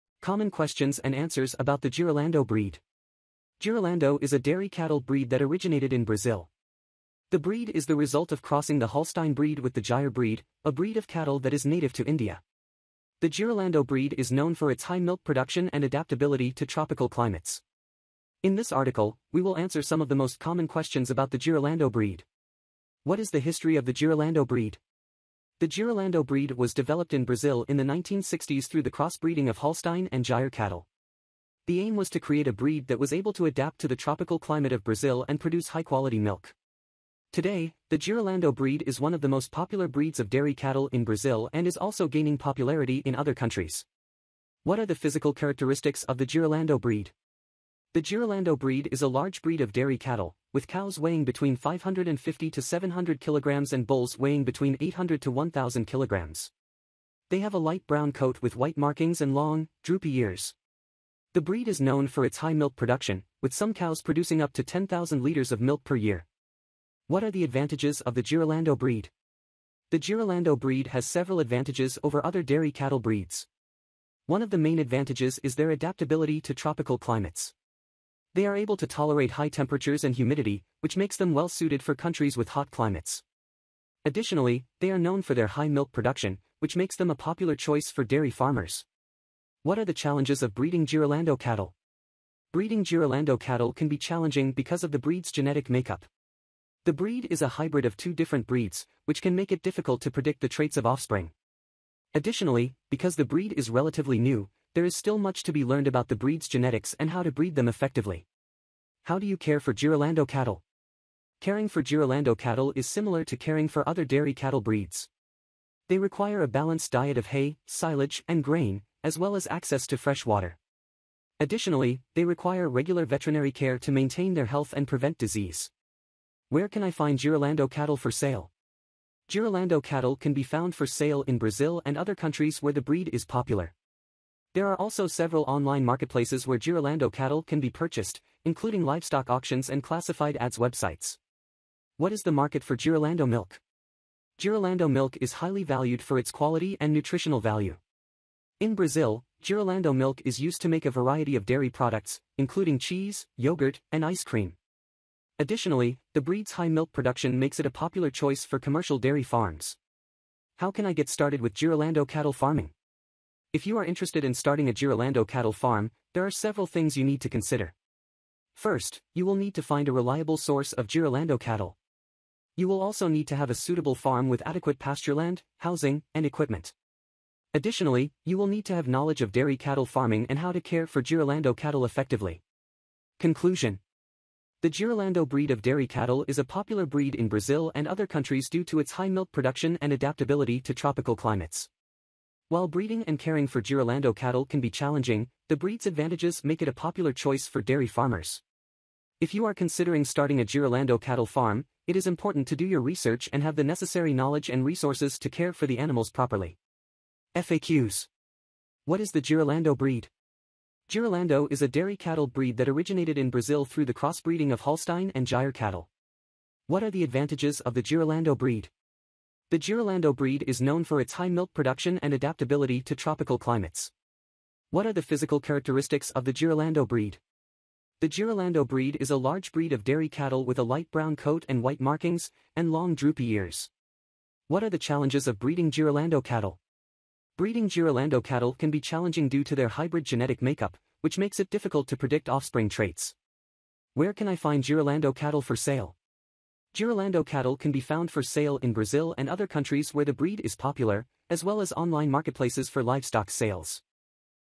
⭕ Common Questions and Answers about the Girolando Breed ✅ Biggest Bulls And Cow
Common Questions and Answers about the Girolando Breed. Girolando is a dairy cattle breed that originated in Brazil. The breed is the result of crossing the Holstein breed with the Gyr breed, a breed of cattle that is native to India. The Girolando breed is known for its high milk production and adaptability to tropical climates. In this article, we will answer some of the most common questions about the Girolando breed.

What is the history of the Girolando breed?.

The Girolando breed was developed in Brazil in the 1960s through the crossbreeding of Holstein and Gyr cattle. The aim was to create a breed that was able to adapt to the tropical climate of Brazil and produce high-quality milk. Today, the Girolando breed is one of the most popular breeds of dairy cattle in Brazil and is also gaining popularity in other countries.

What are the physical characteristics of the Girolando breed?.

The Girolando breed is a large breed of dairy cattle, with cows weighing between 550-700 kg and bulls weighing between 800-1000 kg. They have a light brown coat with white markings and long, droopy ears. The breed is known for its high milk production, with some cows producing up to 10,000 liters of milk per year.

What are the advantages of the Girolando breed?.

The Girolando breed has several advantages over other dairy cattle breeds. One of the main advantages is their adaptability to tropical climates. They are able to tolerate high temperatures and humidity, which makes them well-suited for countries with hot climates. Additionally, they are known for their high milk production, which makes them a popular choice for dairy farmers.

What are the challenges of breeding Girolando cattle?.

Breeding Girolando cattle can be challenging because of the breed's genetic makeup. The breed is a hybrid of two different breeds, which can make it difficult to predict the traits of offspring. Additionally, because the breed is relatively new, there is still much to be learned about the breed's genetics and how to breed them effectively.

How do you care for Girolando cattle?.

Caring for Girolando cattle is similar to caring for other dairy cattle breeds. They require a balanced diet of hay, silage, and grain, as well as access to fresh water. Additionally, they require regular veterinary care to maintain their health and prevent disease.

Where can I find Girolando cattle for sale?.

Girolando cattle can be found for sale in Brazil and other countries where the breed is popular. There are also several online marketplaces where Girolando cattle can be purchased, including livestock auctions and classified ads websites.

What is the market for Girolando milk?.

Girolando milk is highly valued for its quality and nutritional value. In Brazil, Girolando milk is used to make a variety of dairy products, including cheese, yogurt, and ice cream. Additionally, the breed's high milk production makes it a popular choice for commercial dairy farms.

How can I get started with Girolando cattle farming?.

If you are interested in starting a Girolando cattle farm, there are several things you need to consider. First, you will need to find a reliable source of Girolando cattle. You will also need to have a suitable farm with adequate pastureland, housing, and equipment. Additionally, you will need to have knowledge of dairy cattle farming and how to care for Girolando cattle effectively.

Conclusion.

The Girolando breed of dairy cattle is a popular breed in Brazil and other countries due to its high milk production and adaptability to tropical climates. While breeding and caring for Girolando cattle can be challenging, the breed's advantages make it a popular choice for dairy farmers. If you are considering starting a Girolando cattle farm, it is important to do your research and have the necessary knowledge and resources to care for the animals properly.

FAQs.

What is the Girolando breed?.

Girolando is a dairy cattle breed that originated in Brazil through the crossbreeding of Holstein and Gyr cattle.

The Girolando breed is known for its high milk production and adaptability to tropical climates.

The Girolando breed is a large breed of dairy cattle with a light brown coat and white markings, and long droopy ears.

Breeding Girolando cattle can be challenging due to their hybrid genetic makeup, which makes it difficult to predict offspring traits.

Girolando cattle can be found for sale in Brazil and other countries where the breed is popular, as well as online marketplaces for livestock sales.

Power by: Fender Stiv Palencia Gonzalez